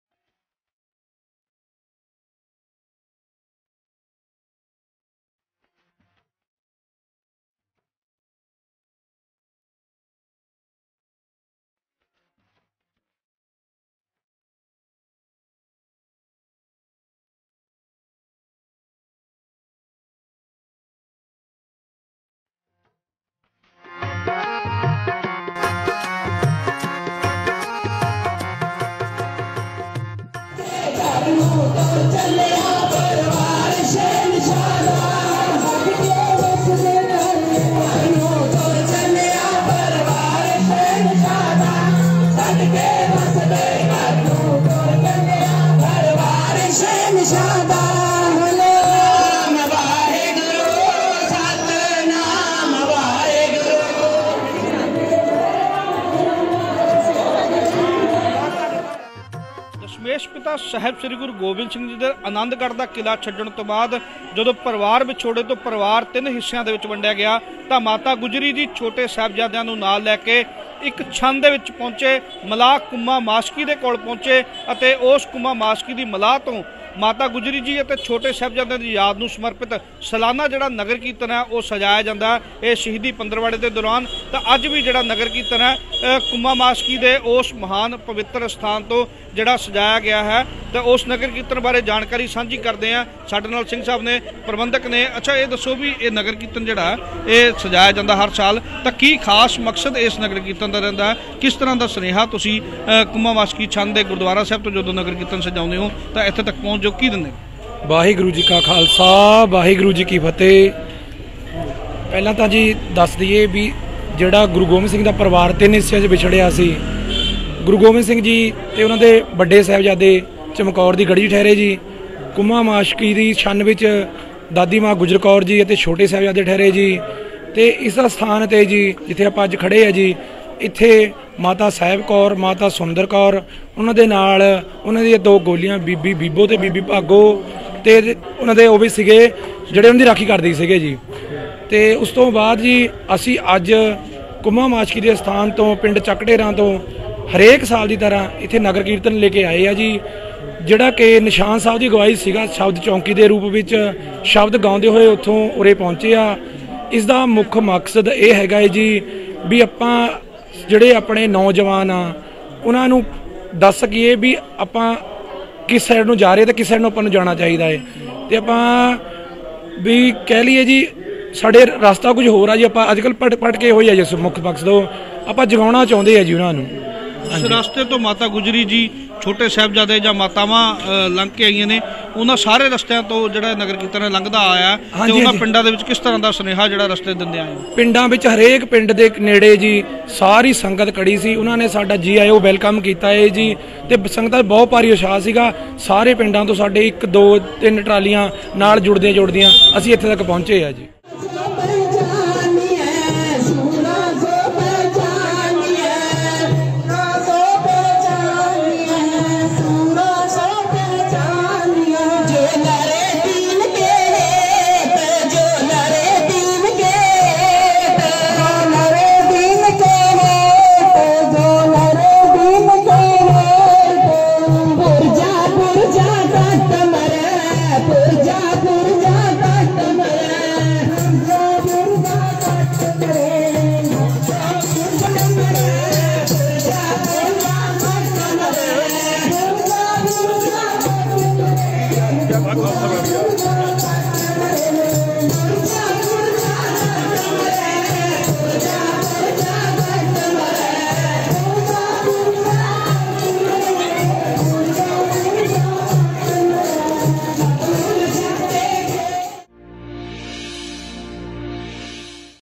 ਸ਼ਹੀਦੀ ਦਿਹਾੜਿਆਂ ਨੂੰ ਸਮਰਪਿਤ ਸਜਾਇਆ ਗਿਆ Nagar Kirtan
ਸ਼ਹੀਦੀ ਦਿਹਾੜਿਆਂ ਨੂੰ ਸਮਰਪਿਤ ਨਗਰ ਕੀਰਤਨ। ਸਜਾਇਆ ਗਿਆ ਸਲਾਨਾ ਨਗਰ ਕੀਰਤਨ। ਵੱਡੀ ਗਿਣਤੀ ਵਿੱਚ ਸੰਗਤਾਂ ਨੇ ਭਰੀ ਹਾਜ਼ਰੀ।